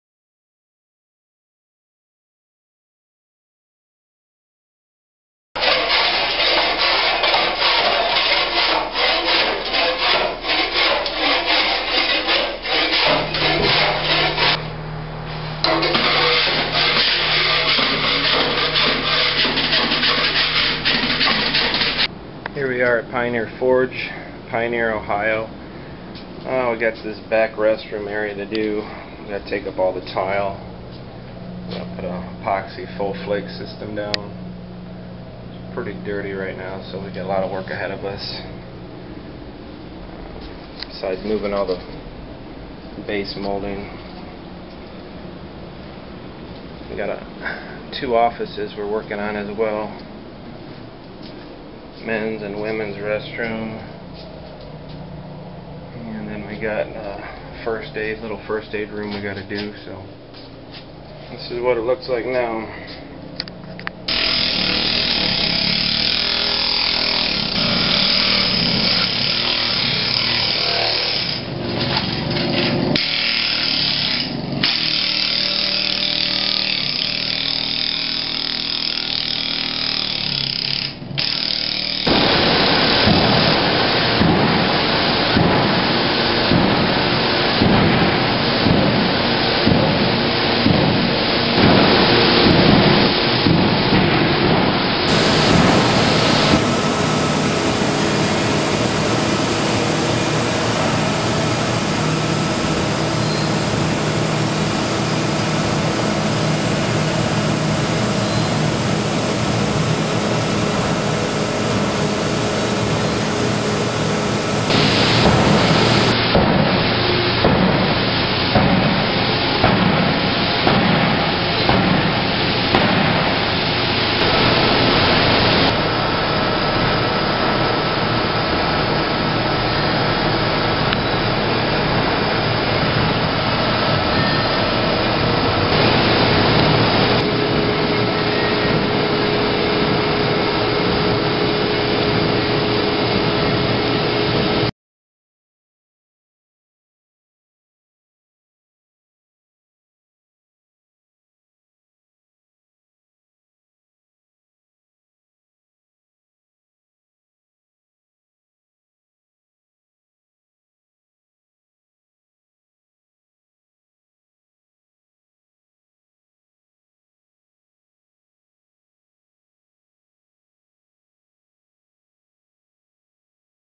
Tile Removal & Epoxy Flake Flooring Replacement | Pioneer, OH
Decorative concrete/epoxy chip project at Pioneer Forge in Pioneer, Ohio. Tile removal of extremely dirty floor and video of finished job.